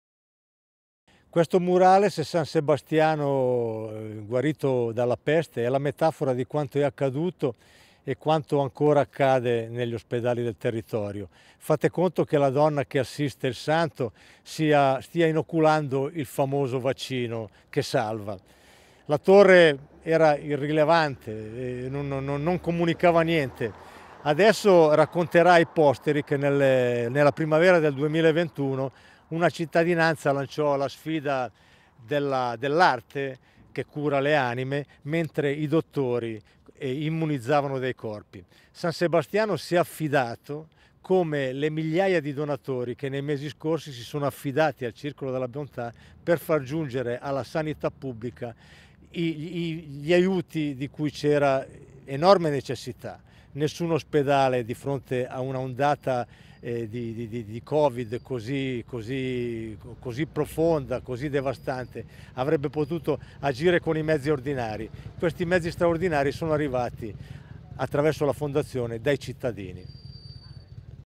Gianni Spartà - Presidente Fondazione Circolo della Bontà presenta il Progetto CurArti
"Perché è nato il progetto CurArti?" L'obiettivo è quello di portare l'arte fuori dagli spazi istituzionali ad essa dedicati e offrirla agli occhi di quanti, ogni giorno, si trovano a trascorrere il proprio tempo in un luogo di cura. Crediamo profondamente nel ruolo terapeutico dell'arte e pensiamo che tra queste mura essa diventi realmente "l'arte di prendersi cura".
Nel racconto del nostro presidente Gianni Spartà ripercorriamo la storia di un progetto artistico che si fa dono per tutta la cittadinanza nel luogo in cui ancora oggi si combatte la battaglia più impegnativa del XXI secolo.

Riprese video e montaggio: Areafilm